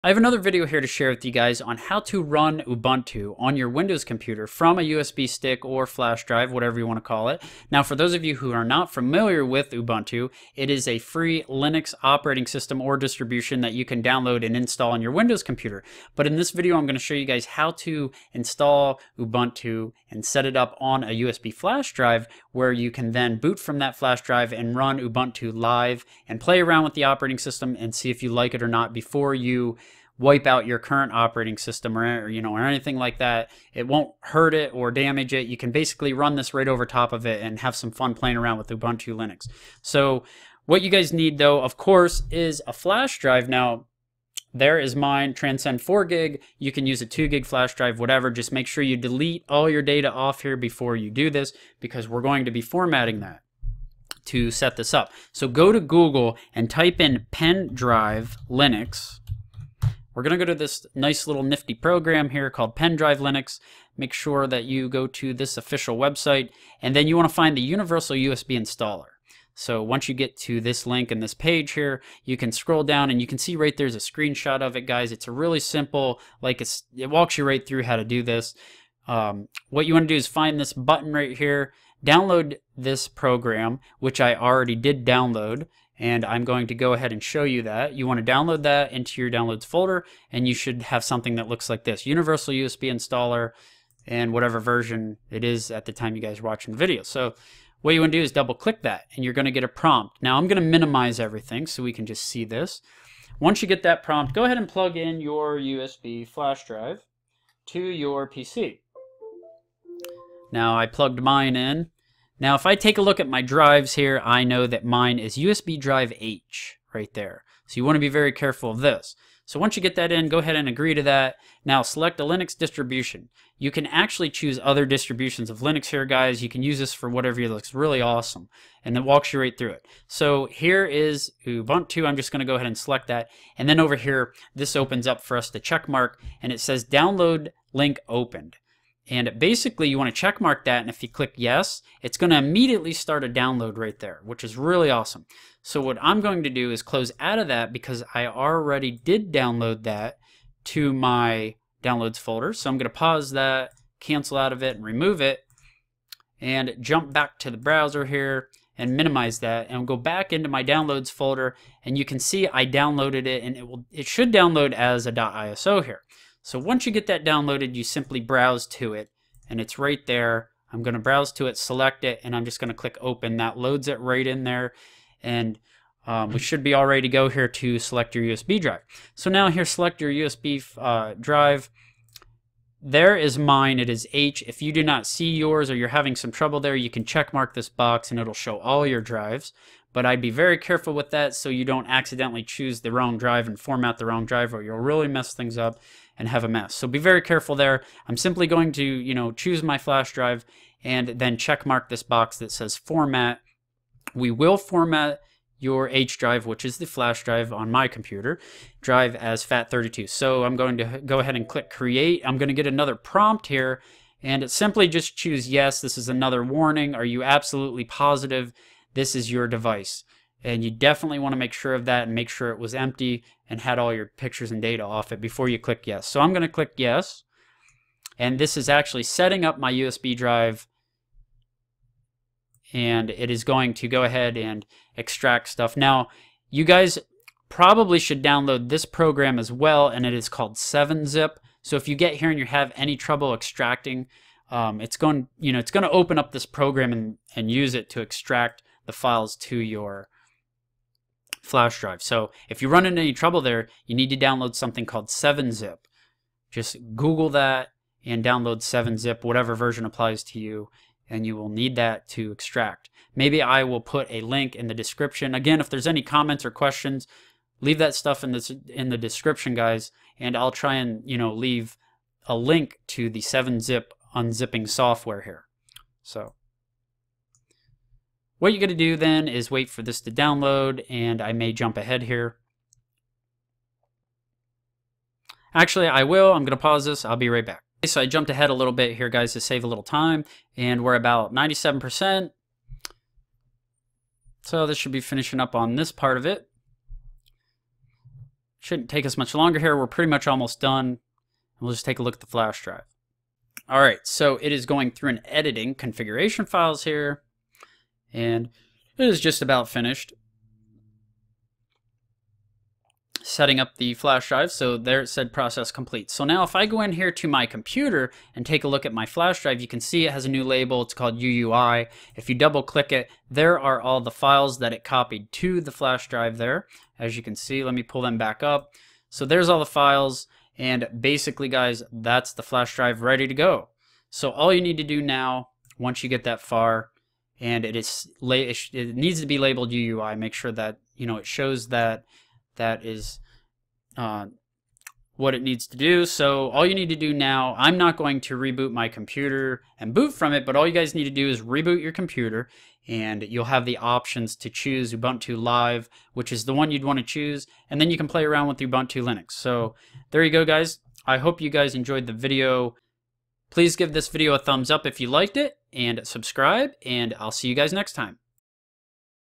How To Install/Create a Bootable Ubuntu USB Stick/Flash Drive
Install/create bootable ubuntu linux usb stick/flash drive, how to run ubuntu live on a windows pc booting from a usb flash drive, create a bootable USB flash drive to install ubuntu or run ubuntu live on a windows computer. Once your usb flash drive is set up you can then restart your computer and boot from that usb flash drive and you will then have the option to choose to run ubuntu live or install ubuntu on your pc. If you choose to run ubuntu live option you can use and play around with ubuntu, without wiping out or formatting your current windows operating system.